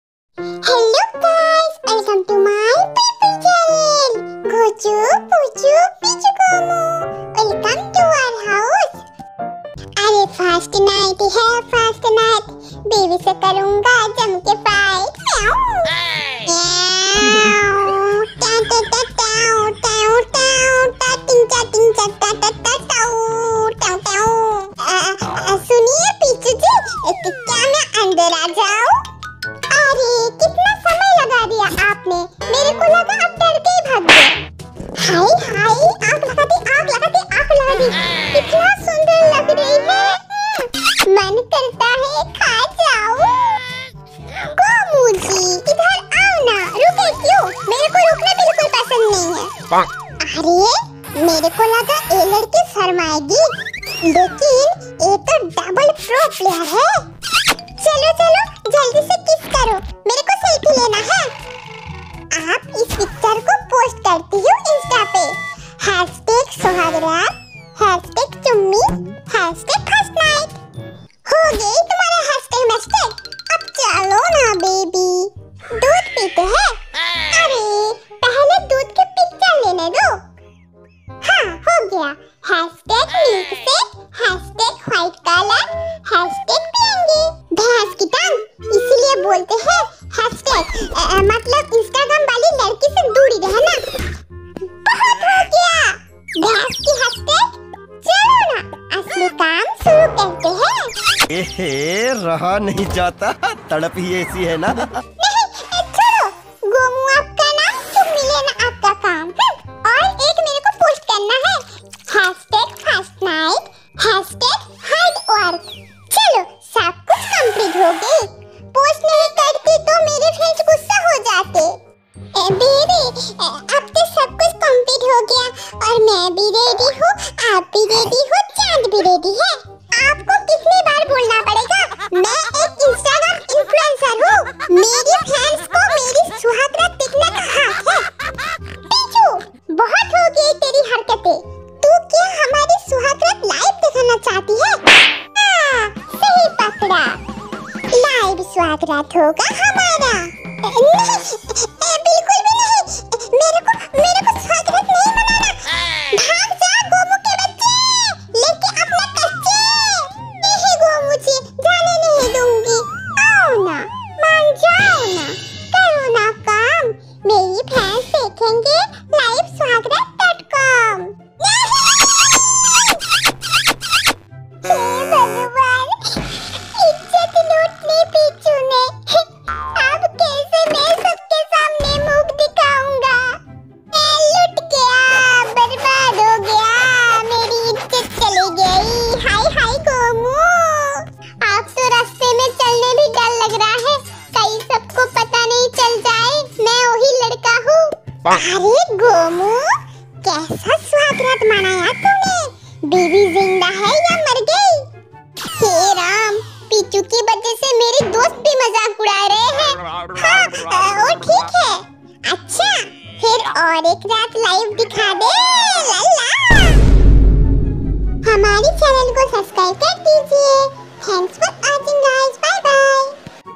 Live Suhagrat 🤤😍❤️ With Peachu Gomu 😂 Full Funny Story|Mou Das|Bubu Dudu Funny Story|Bear Panda
This Video Family Ke Sath Na Dekhe🙏
Bache Na Dekhe 🙏

Aise Insta Par Youtube Par Bache Chahe to bohut Kuch Dekh Sakte ha Iske liye koi kuch nhi bolega but meri video mein gande comments a sakte ha isiliye sorry

maine views ke liye koi videos nhi banaya funny ke liye banaya❤️agar kisiko Gyan Dena ho to insta bale company ko Gyan do Jo isse jada gande asob video make karte ha.